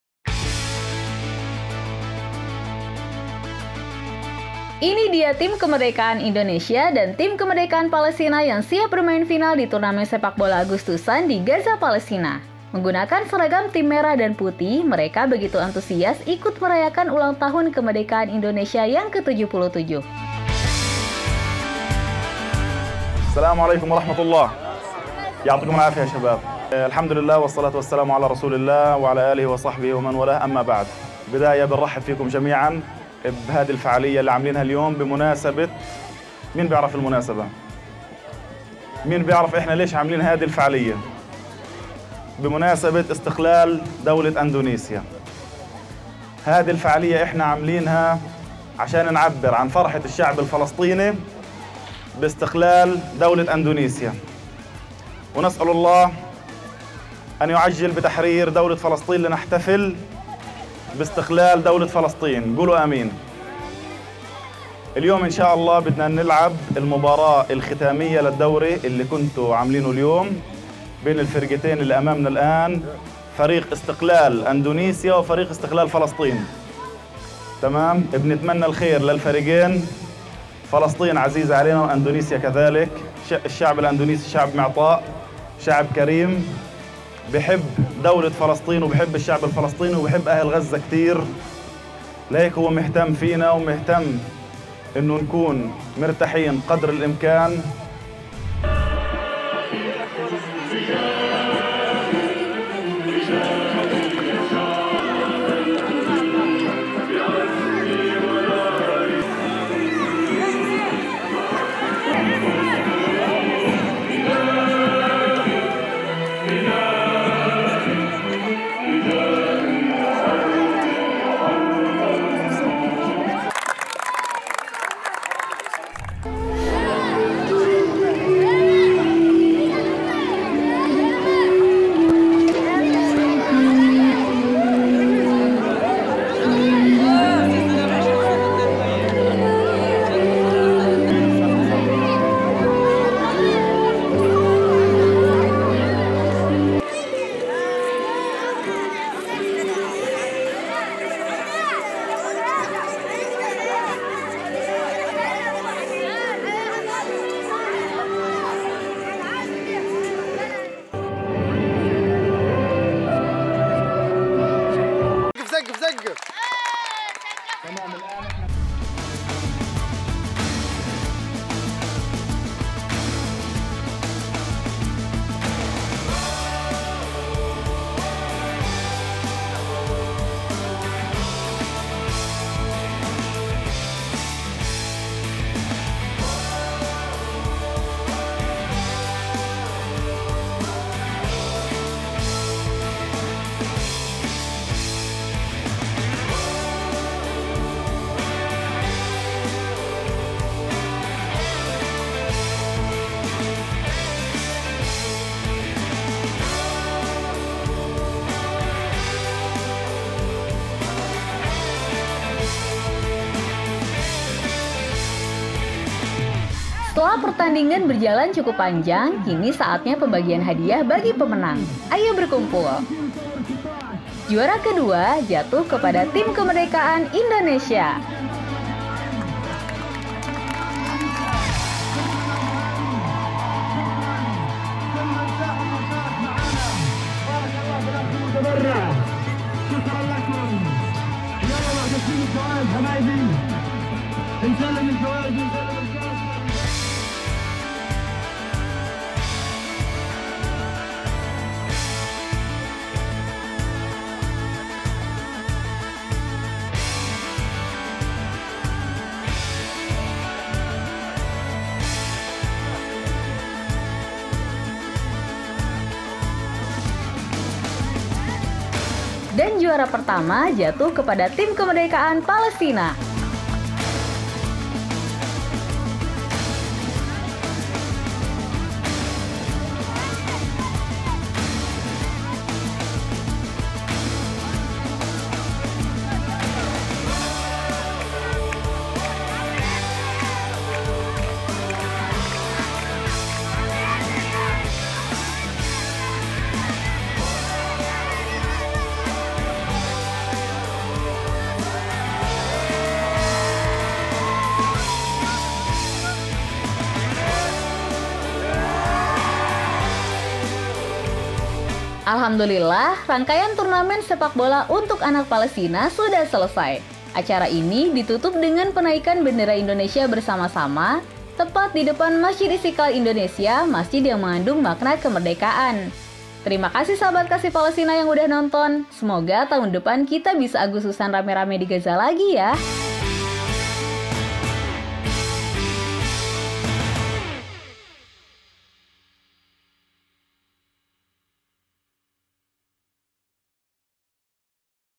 SIAPA YANG MENANG? Final Turnamen Sepakbola Agustusan di Gaza Palestina
Ini dia keseruan final turnamen sepakbola Anak-anak Palestina di Gaza.
Final ini jadi akhir dari rangkaian lomba-lomba agustusan yang digelar di sekitar Masjid Istiqlal Indonesia.

Masya Allah.. Seru banget ya..